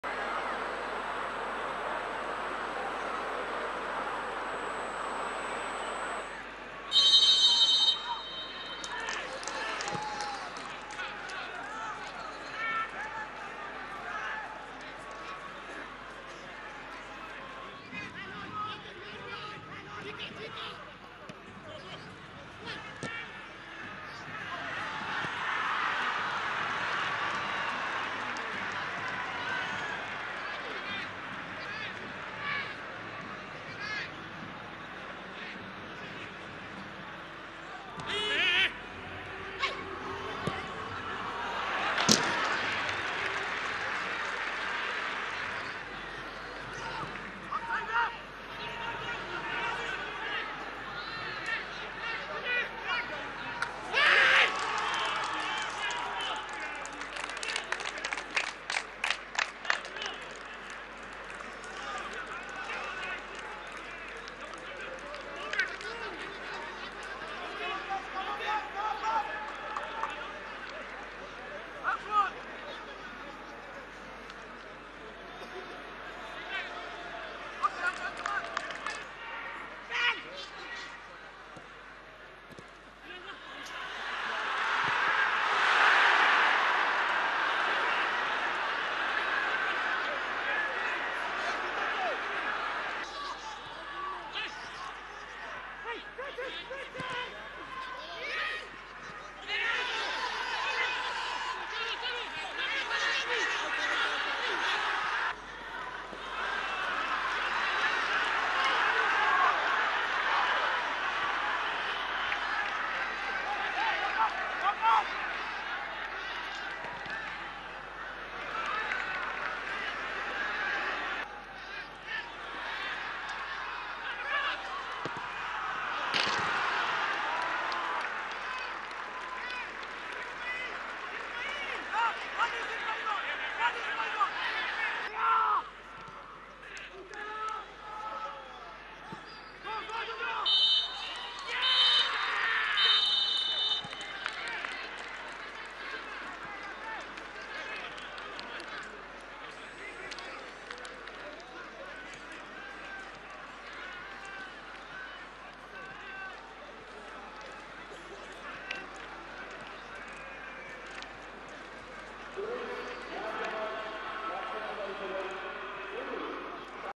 Mohammedan Sporting vs Salgaocar (Match Highlights) - Feb 22, 2014 [0-1]
Kolkata: A first-half strike from Dudu Omagbemi saw Salgaocar beat Mohammedan Sporting 1-0 in the round 18 encounter of the Airtel I-League at the Salt Lake Stadium on Saturday evening.